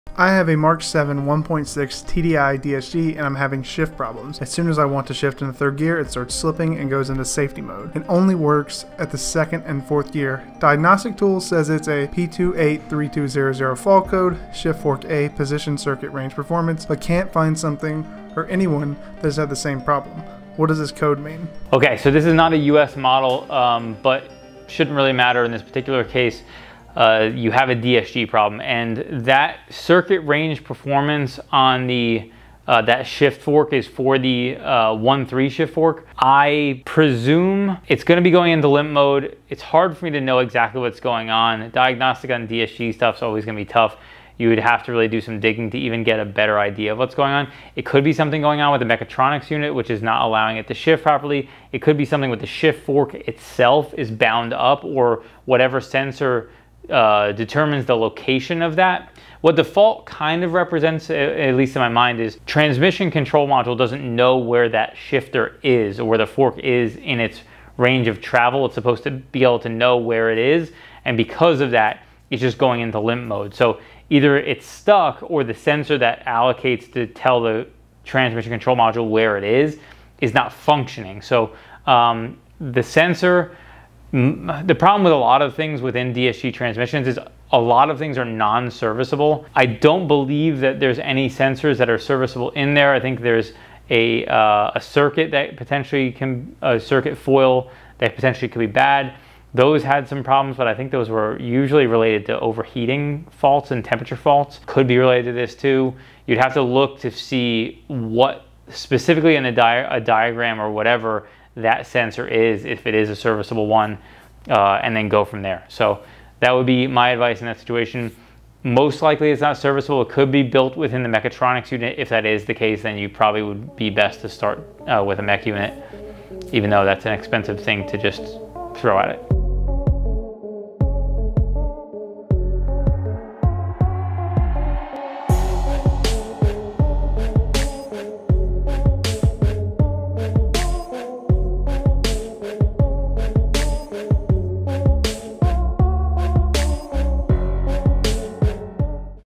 Transmission Shift Fork Malfunction | P283200 | AskDap
Q - ko4572ko - via Instagram - I got an mk7 1,6 TDI (DSG) and I'm having shift problems...as soon as it wants to shift into the 3rd gear it starts slipping and goes into "safety mode" and only works at the 2nd and 4th gear...diagnostic tools says its a p283200 fault code Shift Fork 'A' Position Circuit Range/Performance but can't find something or anyone that has had the same problem? What does this code mean?

This video was shot on location:
Deutsche Auto Parts Inc.
10308 Bailey Rd
Suite 417